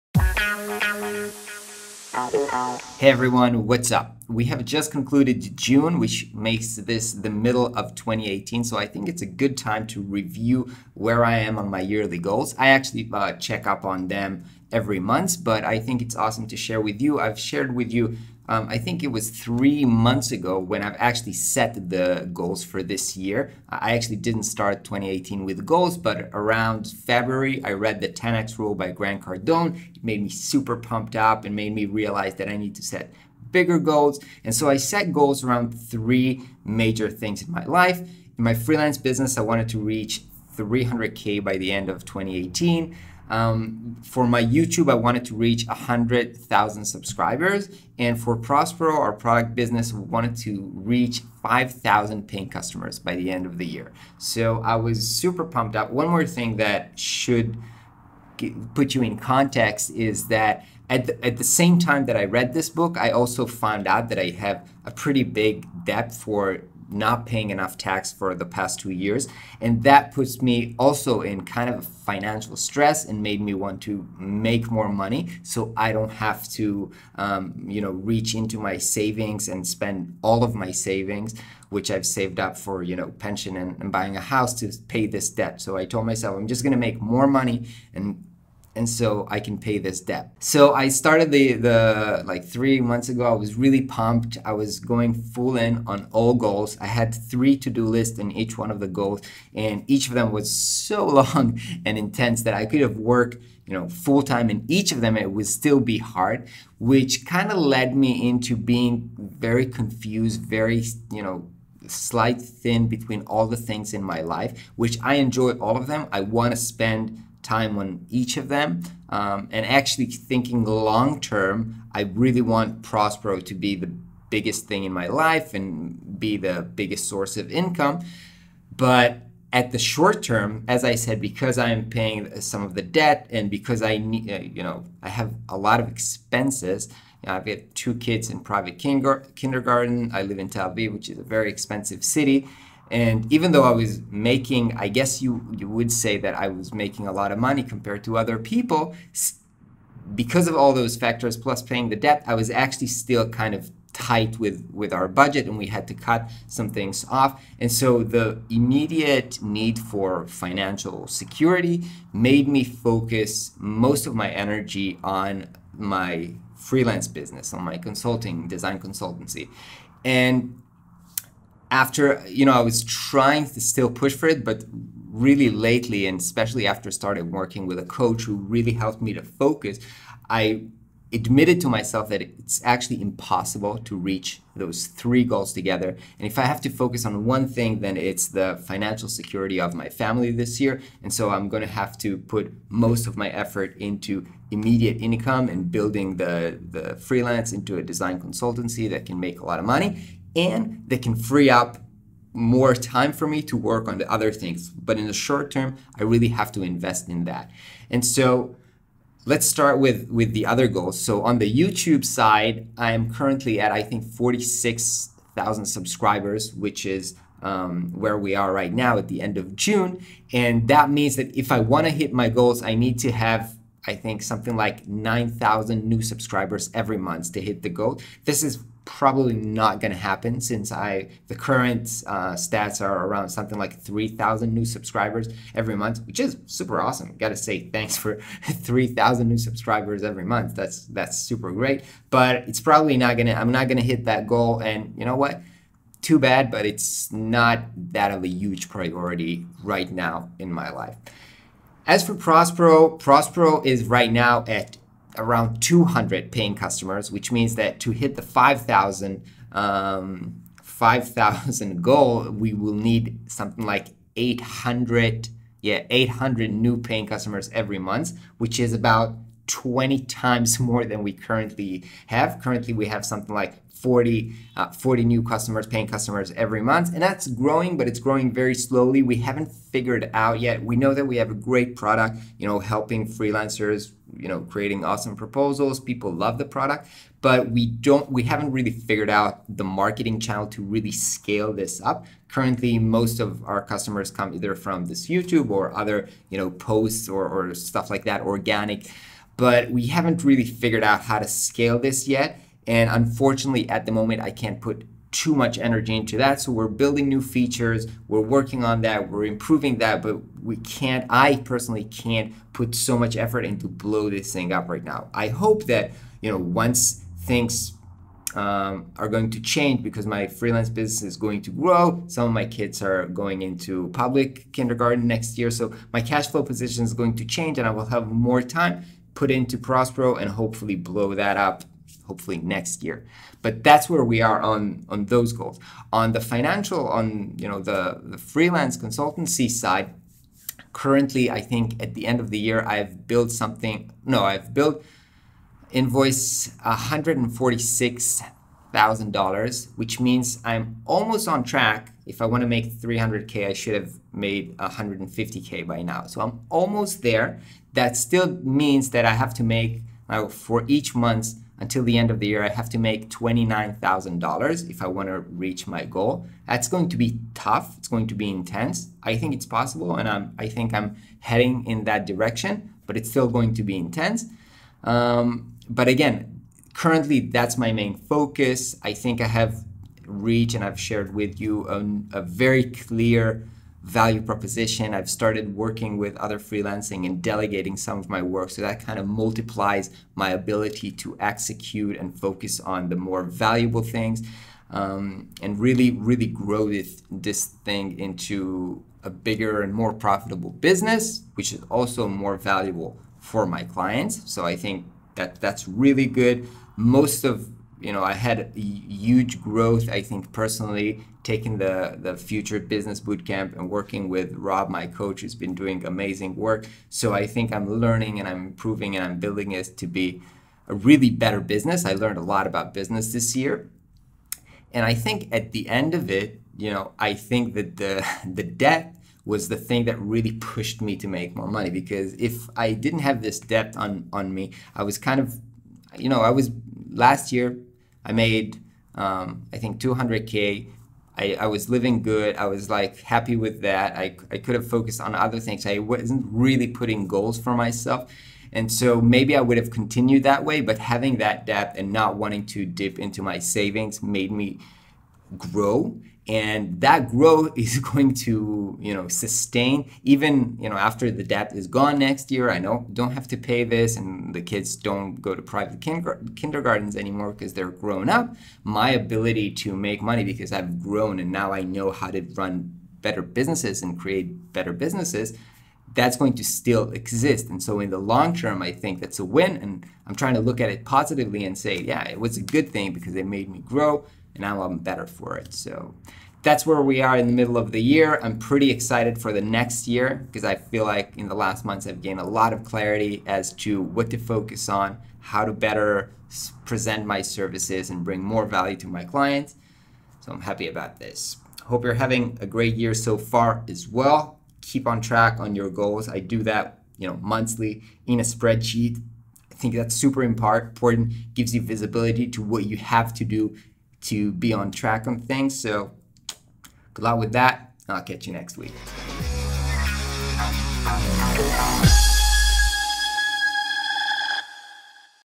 Mid-Year Goals Review 2018
I started the year with 3 goals around freelancing, YouTube, and Prospero. Here's where we are today.